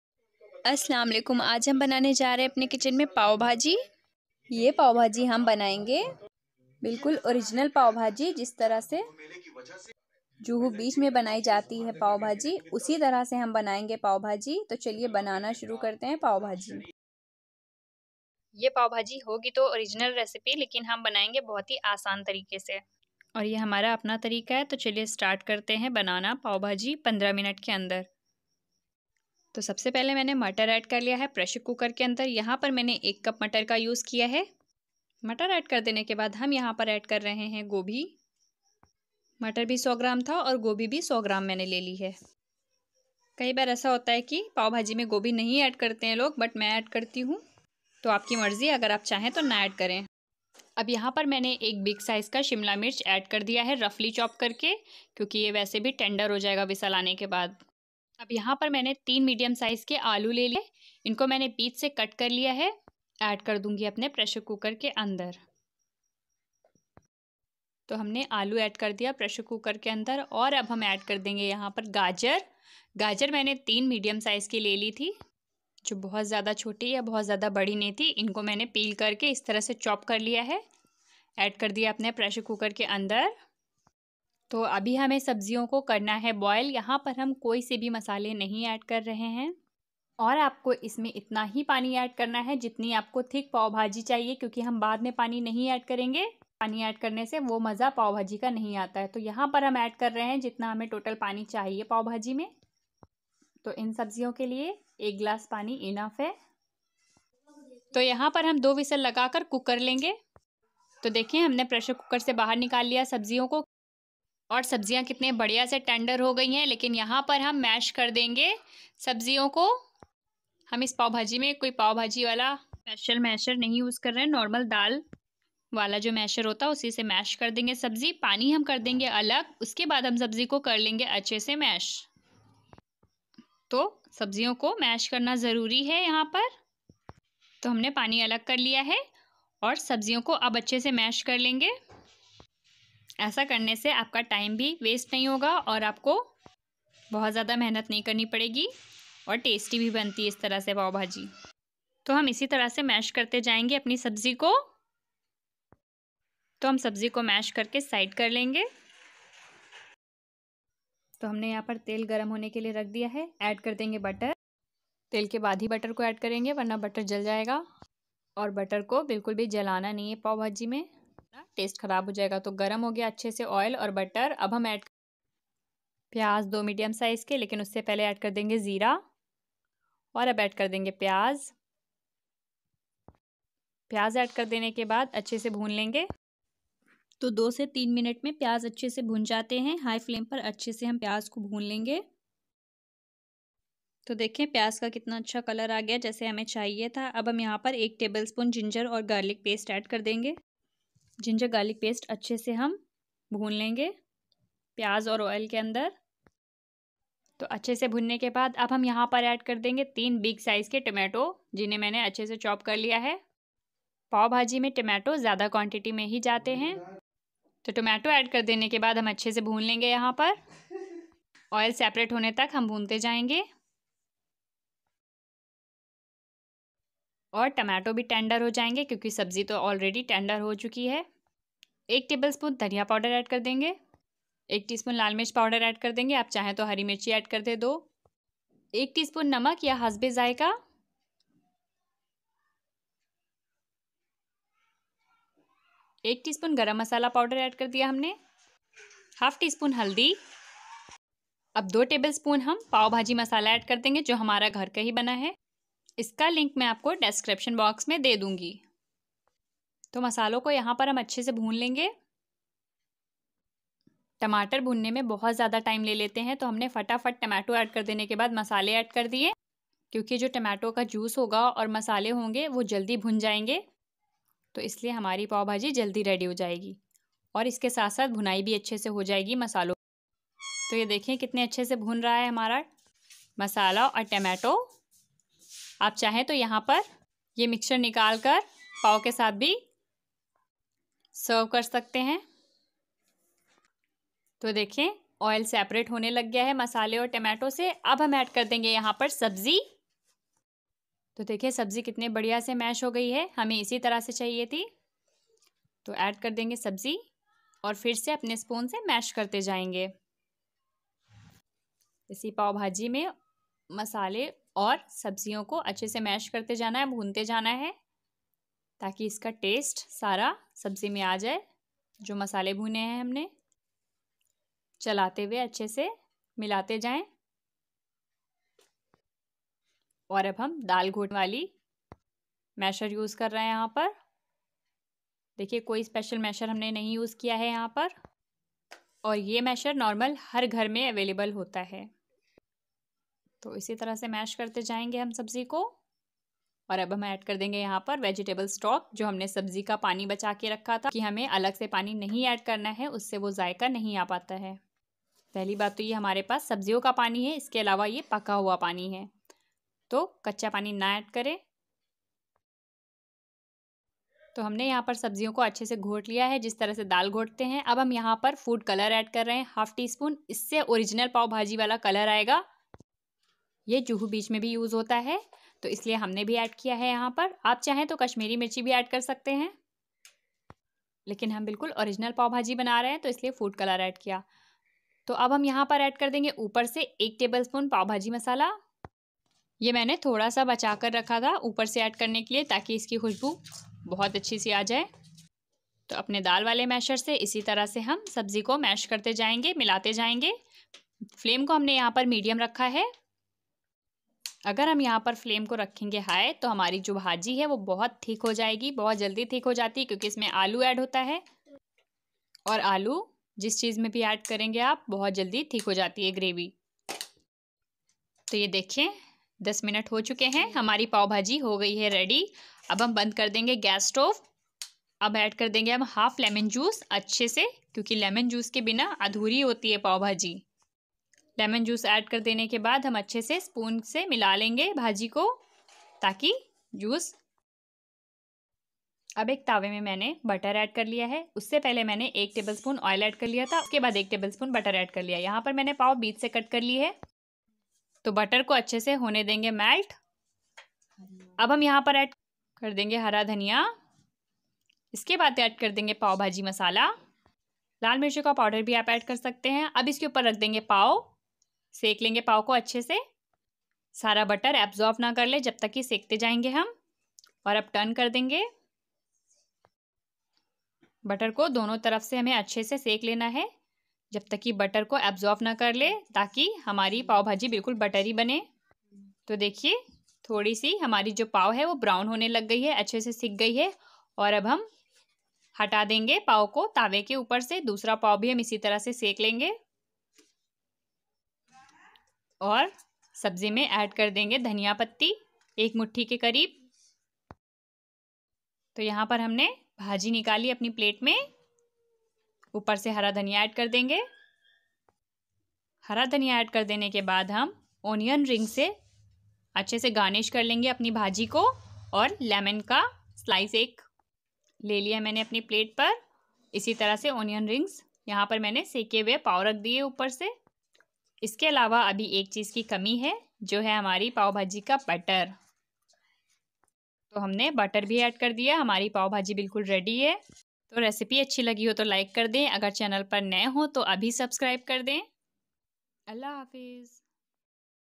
Original juhu beach pavbhaji recipe minto me ban jane vali asan tarike se no special masher no tava
Quick recipe